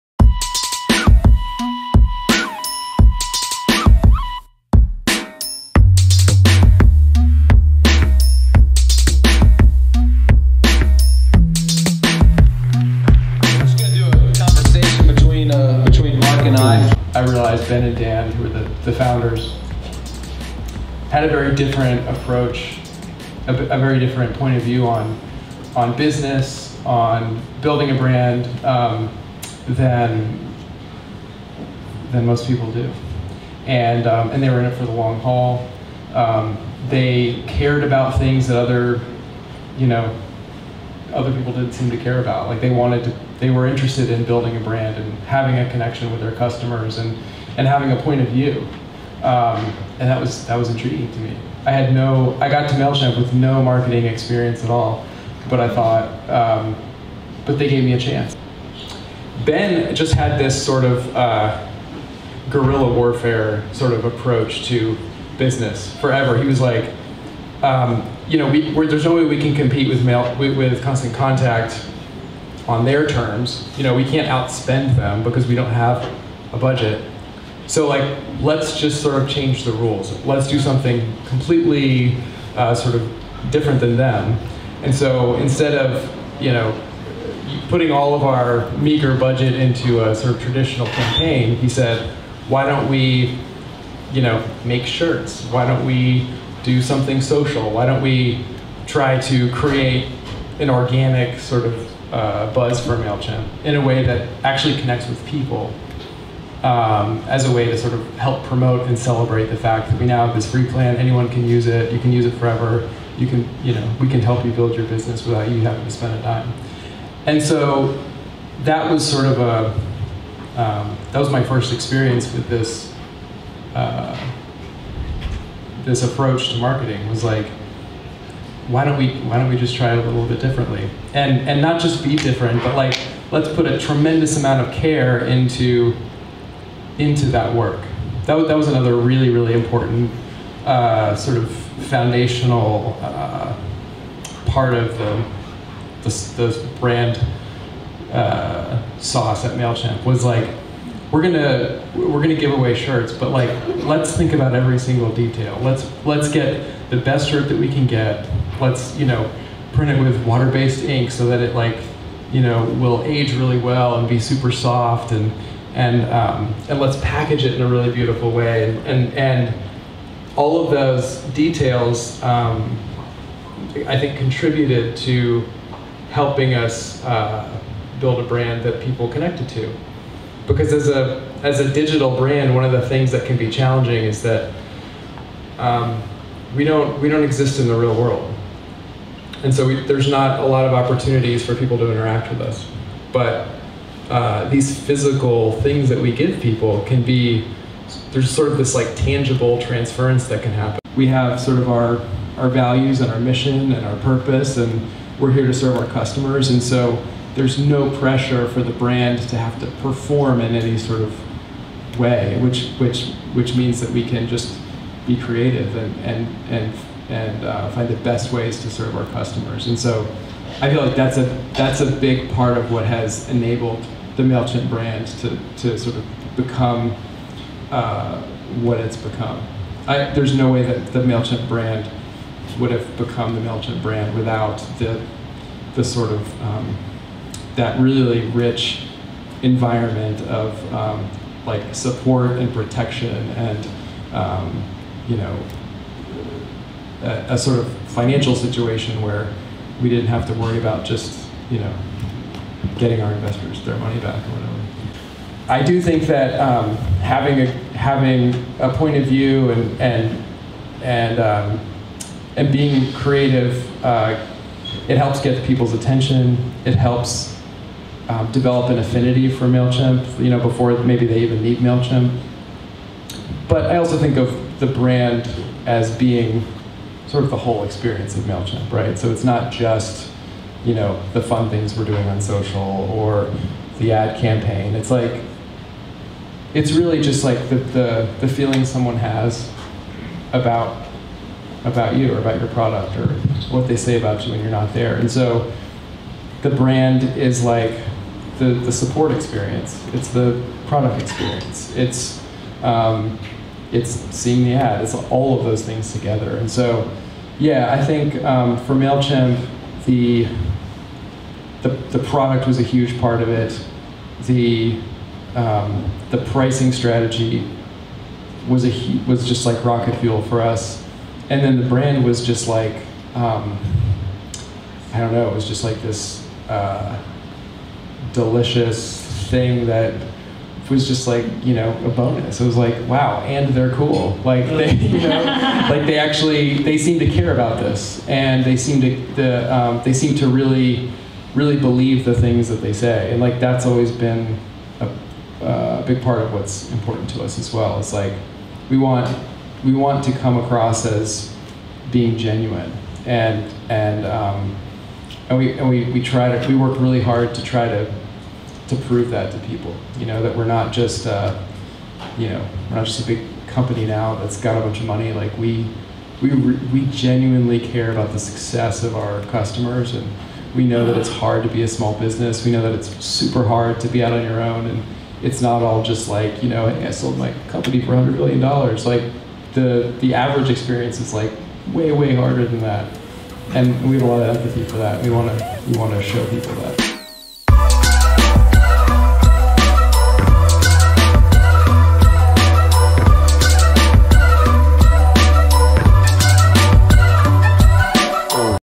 The Mailchimp Brand - Made in Atlanta
From humble beginnings Mailchimp has grown into Atlanta's biggest and best Unicorn. Mark DiCristina is the Sr Director of Brand Marketing at Mailchimp and has been with the company since the early days. Mark explains how Mailchimp used brand as a product differentiator as well as a driver of culture.